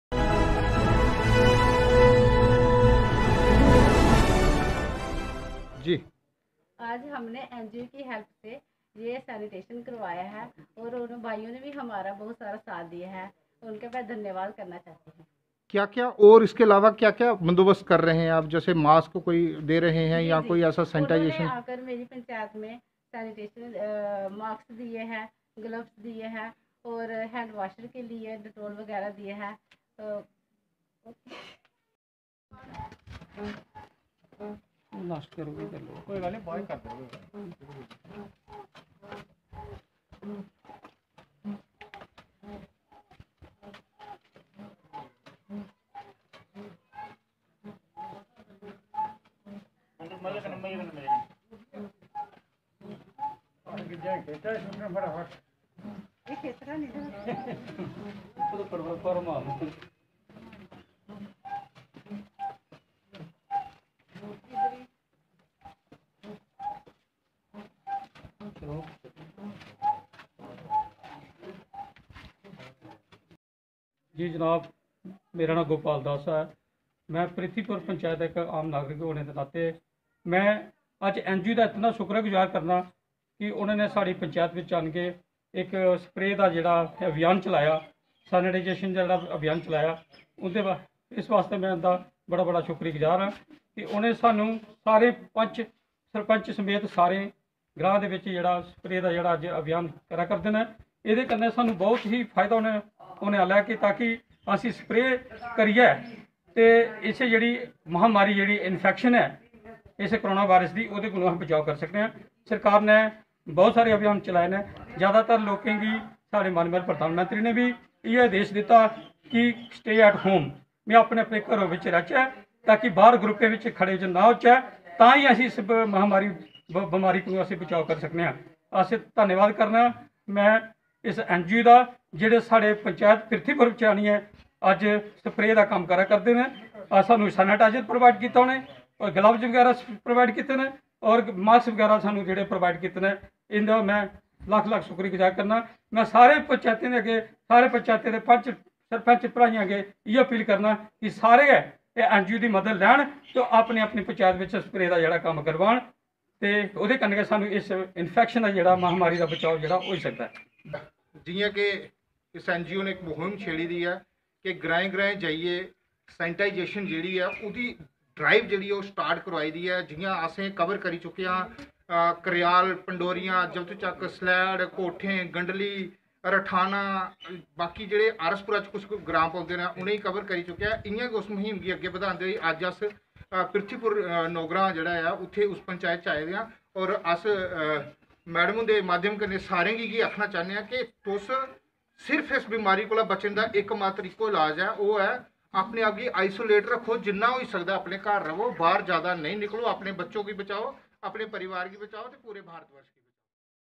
JK First News, In continue reach in the rural area Awareness cum sentization drive on Covid -19 th
In continue reach in the rural area Awareness  cum sentization drive on Covid -19 the team of Nehru Adarsh Youth Club Salehar is Visit at village Prithi Pur Nogran Panchyat Halqa block Bishnah . The volunteer were present with smt Sudes kumari Sarpanch and Panchs of  Panchyat. The team of Nehru Adarsh Youth Club taking instant action and senitize the village and cover all  4 villages under this Panchyat and also aware about mantain social distance.